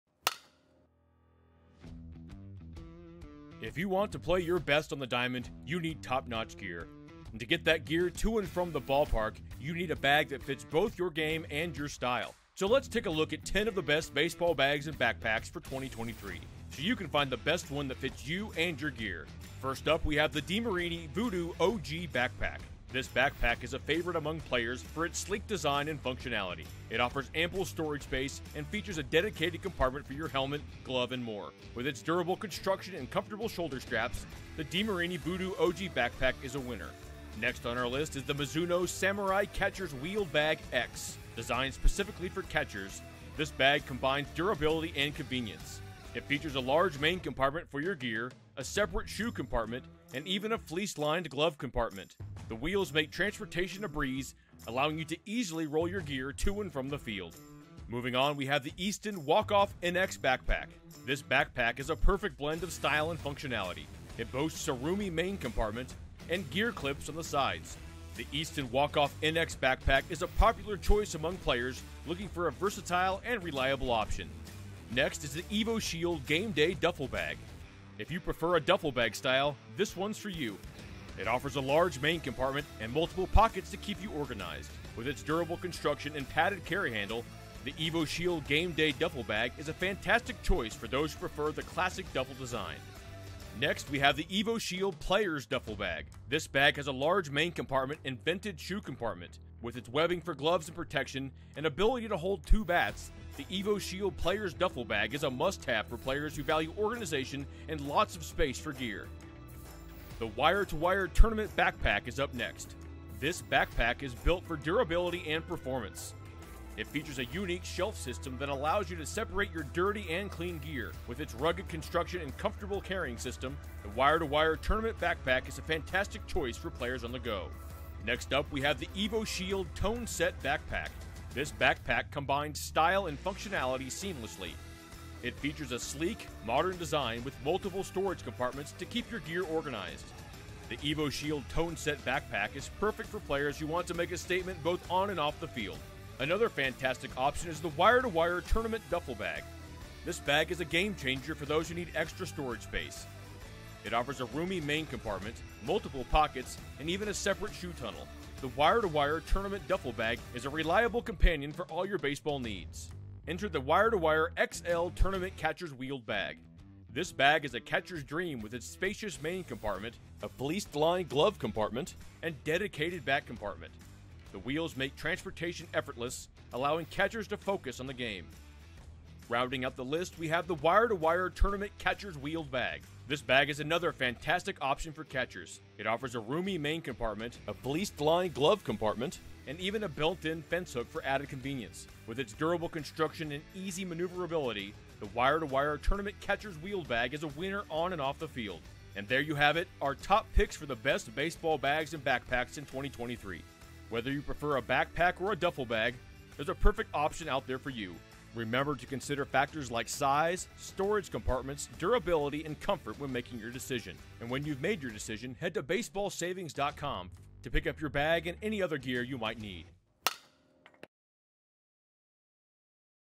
Baseball Savings' Best Baseball Bags of 2023
Baseball Savings rounds up our picks for the best baseball bags and backpacks for 2023.